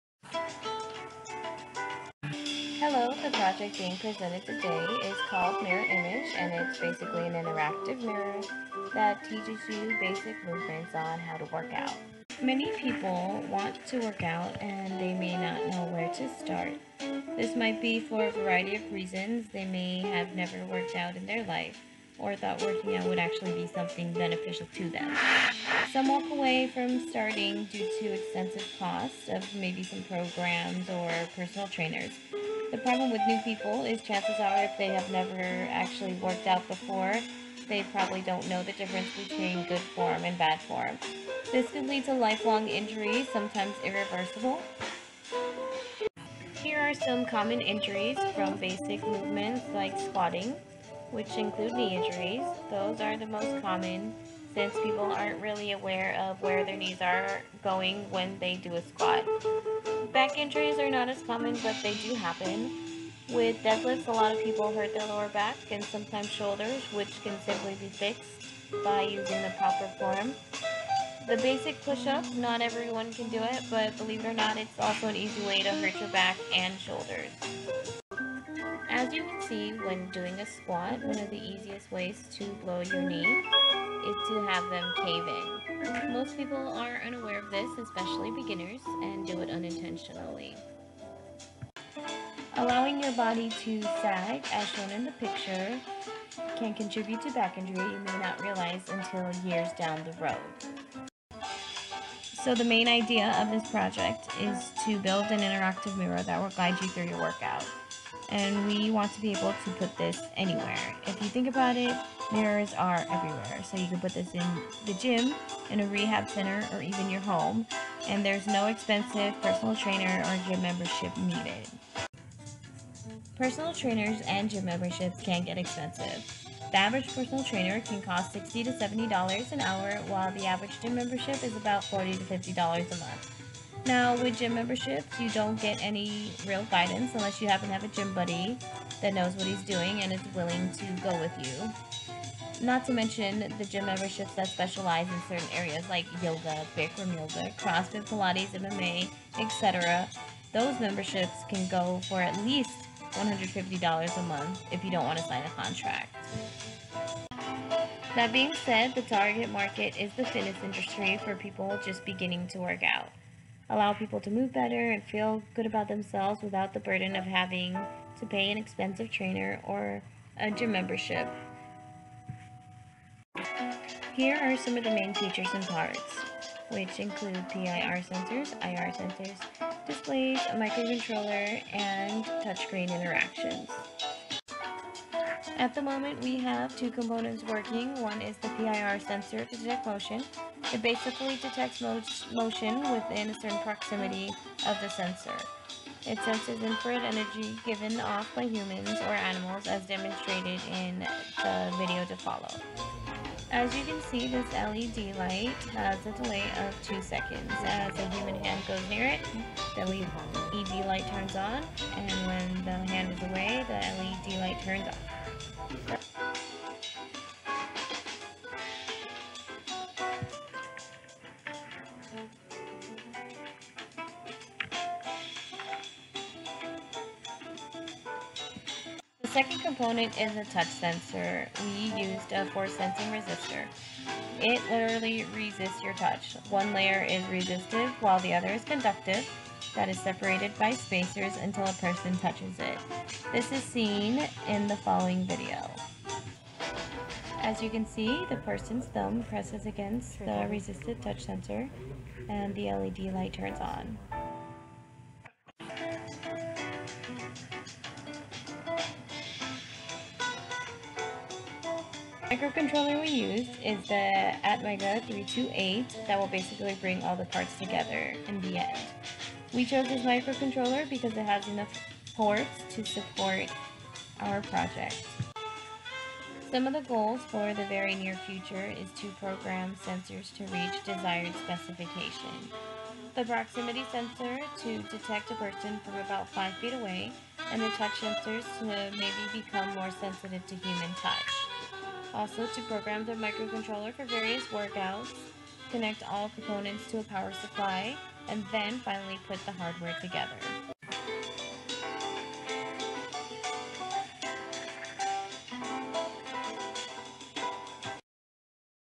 Video Mirror Image
This project is created to help people perform their exercises properly in the comfort of their homes without having to go to the gym and pay for a trainer. For this project, we must test out each parts that are wanted in the project separately. Since the power supply for the project will be an outlet, a circuit is designed to distribute the power needed to make all the components work. Finally, image processing is done to display the trainer and the workouts the person can choose from. Once all three components are completed, it is all brought together to create the final product of our project.